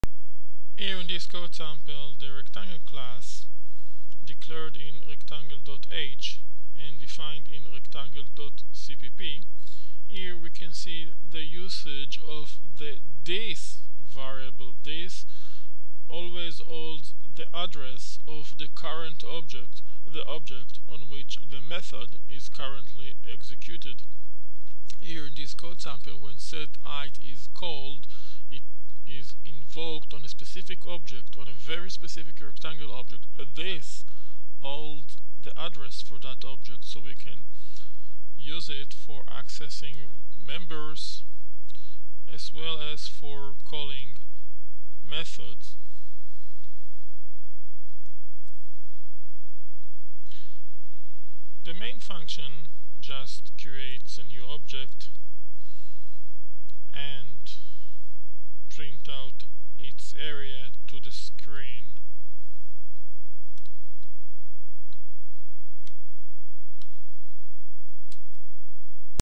The this Pointer in C++
Short video clip that explains the usage of the 'this' pointer in C++.